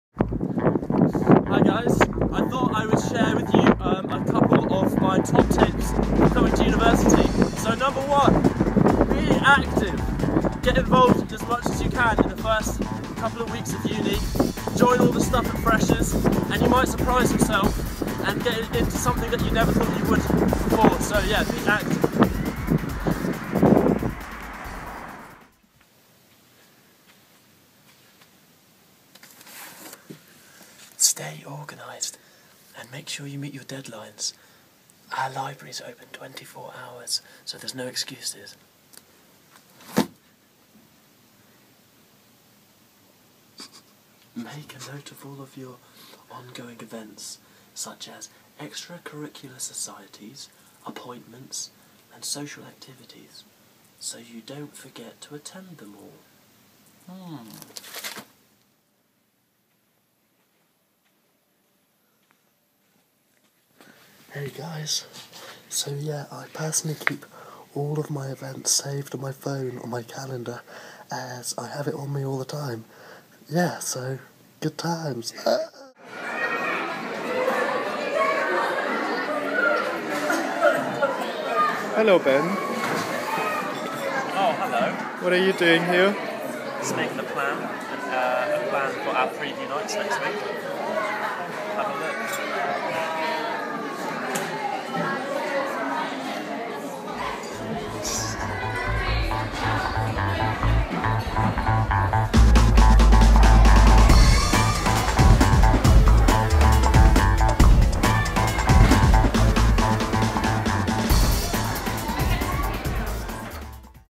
Kingston University Drama student Ben shares a few of his top tips for life at uni
Drama student and Kingston University Student Ambassador Ben shares a few of his top tips for success at university.